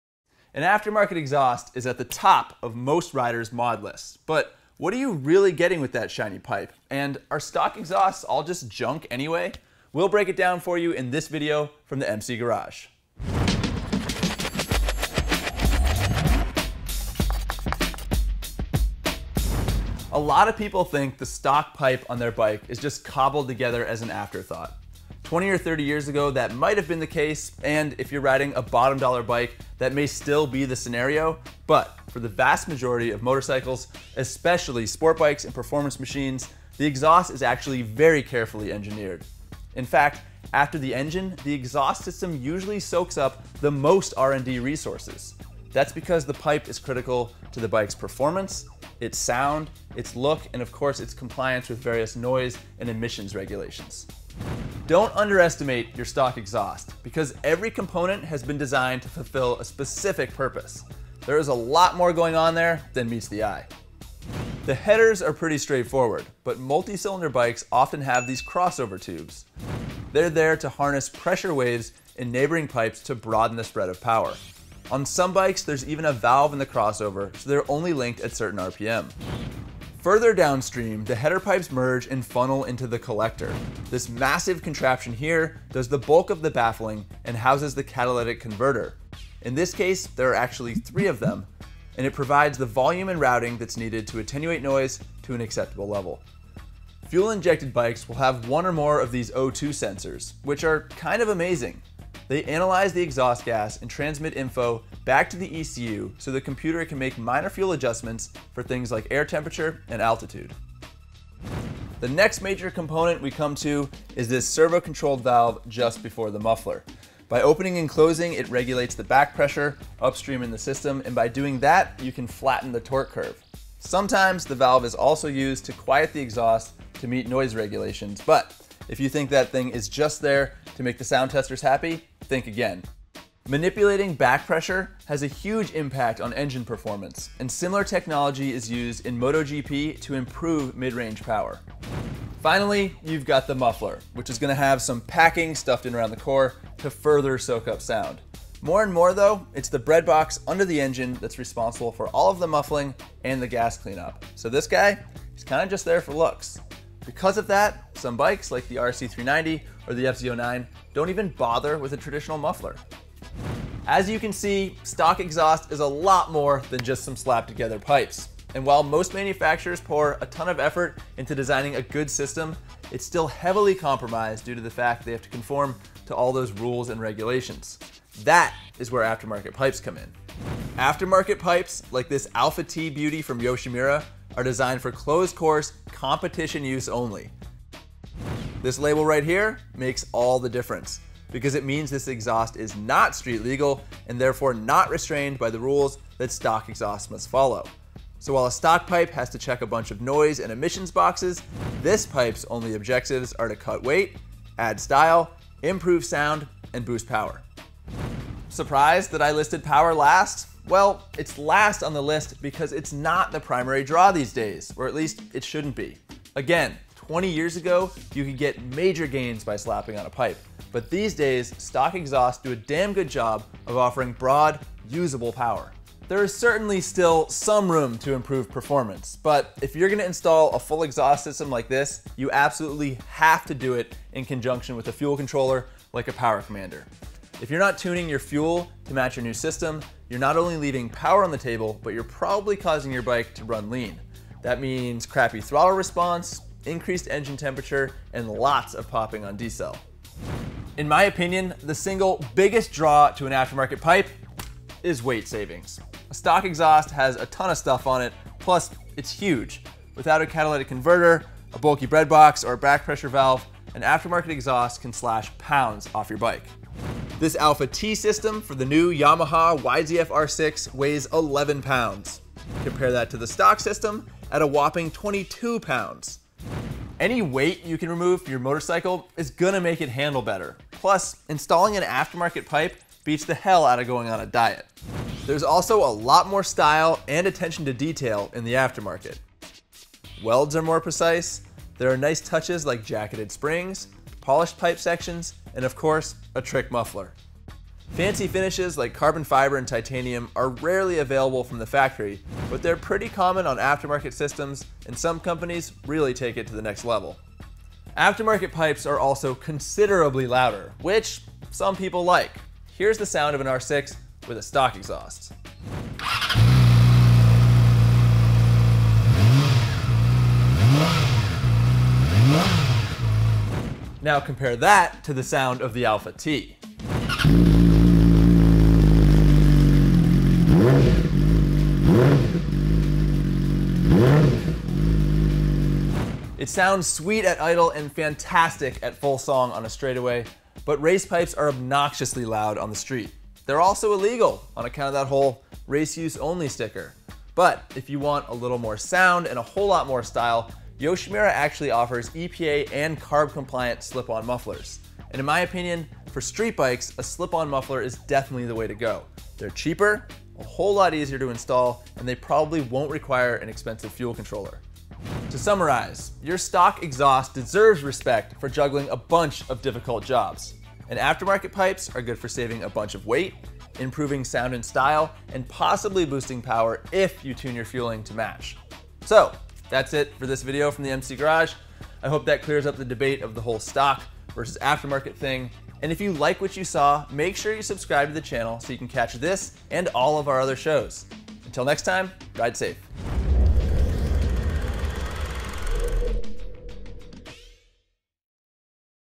The Truth About Aftermarket Exhausts | MC Garage
An aftermarket exhaust is at the top of most rider’s mod list, but what can you really expect from that fancy pipe, and are stock exhausts all just junk anyway? Ari explains in this video from the MC Garage.

An aftermarket exhaust system is at the top of most rider's mod list, but what can you really expect from that fancy pipe, and are stock exhausts all just junk anyway?

A lot of people think the stock pipe on their bike is just cobbled together as an afterthought. Twenty or thirty years ago that might have been the case, and, if you're riding a bottom-dollar bike that may still be the scenario, but for the vast majority of modern bikes, especially sportbikes and performance machines, the exhaust is very carefully engineered. In fact, after the engine, the exhaust system usually soaks up the most R&D resources. That's because it's key to the bike's performance, sound, appearance, and of course compliance with various emissions and noise regulations.

Don’t underestimate that stock exhaust, because every component has been carefully designed to fulfill a specific purpose, and there’s a lot more going on than meets the eye. Most manufacturers pour a ton of effort into designing a good system that provides strong power across the rev range, but stock exhausts are still heavily compromised due to the US and European noise and emissions regulations they must meet. And that is where aftermarket exhausts come in.

Aftermarket pipes are designed for closed course, competition use only. That means such exhaust are not street legal and therefore not restrained by all the rules that stock exhausts must follow. So while your bike’s stock pipe has to check a bunch of noise and emissions boxes, aftermarket exhausts’ only objectives are to cut weight, improve sound, add style, and boost power.

Surprised that I listed power last? It’s last because it isn’t the primary draw these days, or at least it shouldn’t be. Again, twenty years ago you could get big gains simply by slapping on a pipe, but these days, stock exhausts do a damn good job of offering broad, useful power. There is certainly still some room to improve performance, but if you install a full exhaust system, you absolutely have to do it in conjunction with a fuel controller like a Power Commander. If you’re not tuning your fuel to match your new system, you’re leaving power on the table and probably causing your bike to run lean. That means crappy throttle response, high engine temps, and lots of popping on decel.

In my opinion, the single biggest draw of an aftermarket pipe is weight savings. There’s a lot of crap on stock exhausts, and they’re huge! With no heavy catalytic convertor or massive sound-absorbing bread box or backpressure valve, race pipes can slash pounds off your bike.

There’s also a lot more style and attention to detail in the aftermarket, so swapping to a race pipe can turn your exhaust from an ugly necessity to a point of pride. Welds are more precise, there are nice touches like jacketed springs, polished pipe sections, and of course a trick muffler. Fancy finishes like carbon fiber and titanium are rarely available from the factory, but they’re pretty common on aftermarket systems and some companies really take it to another level.

Aftermarket pipes are louder, too, which some people like. I think race pipes sound obnoxious on the street, and they’re also illegal on account of that whole “competition use only” label. But if you’re still after a little more volume and better tone plus a dose of style, Yoshimura has a whole line of EPA- and CARB-compliant mufflers. And honestly, a slip-on muffler is going to be the best option out there for streetbikes. They’re cheaper, much easier to install, and in most cases won’t require an expensive fuel controller.

So, to summarize, your stock exhaust deserves respect for juggling a lot of difficult jobs, and aftermarket exhausts are good for reducing weight, improving sound and style, and possibly boosting power if you optimize your fueling to match it.